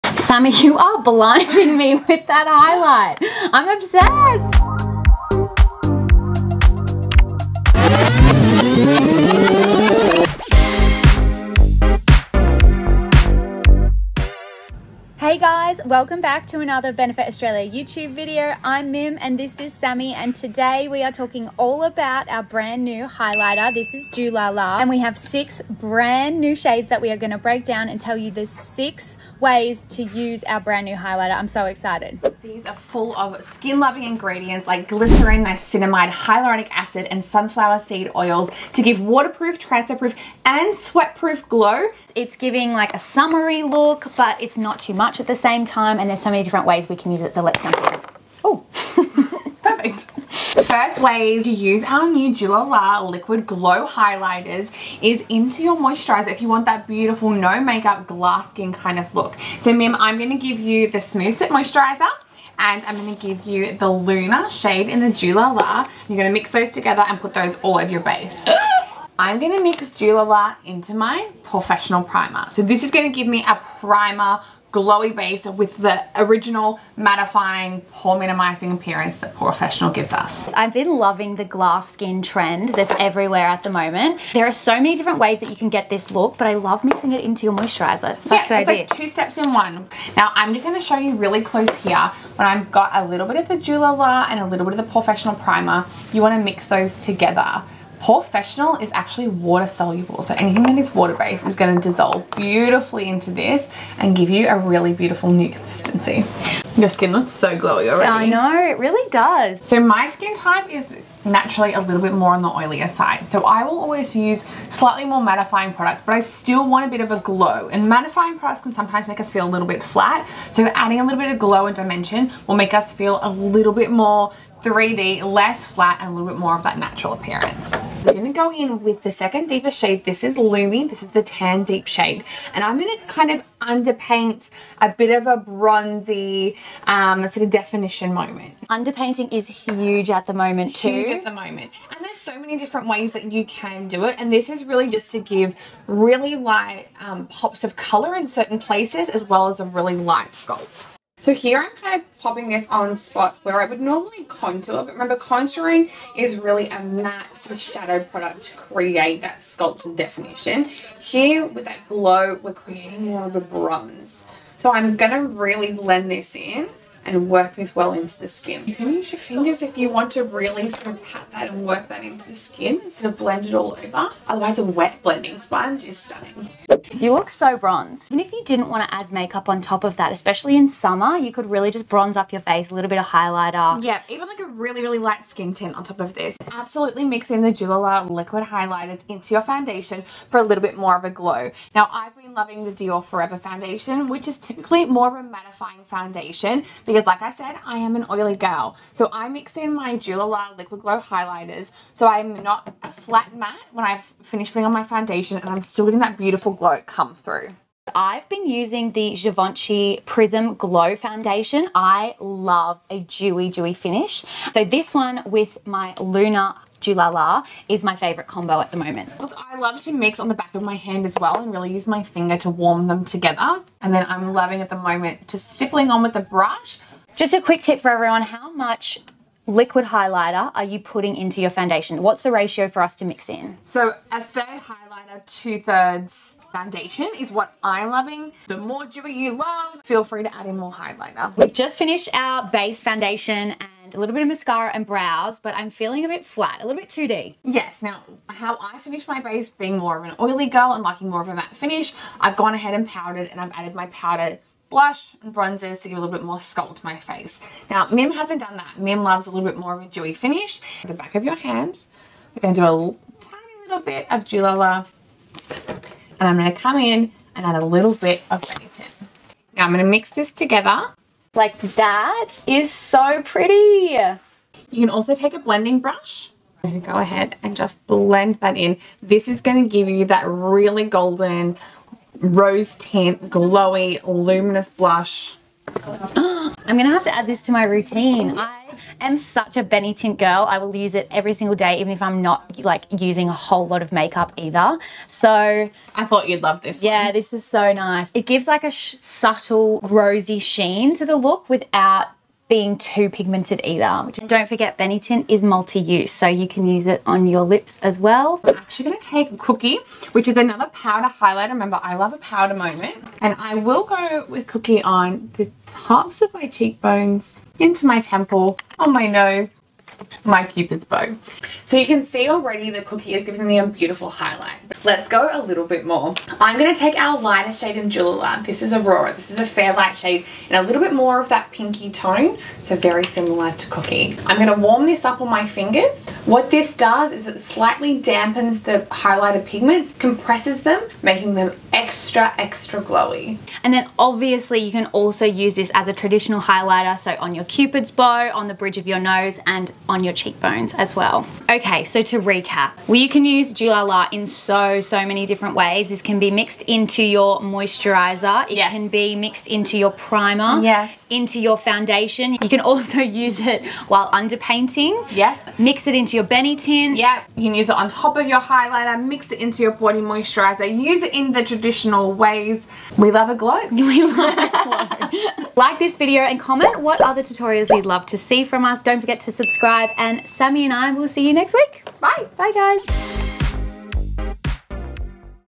6 Ways to Use Our NEW Liquid Highlighter, Dew-La-La!
Hey Benefam! Sammy and Mim here, we're back with another video and this time it's all about our brand NEW Liquid Highlighter, Dew-La-La!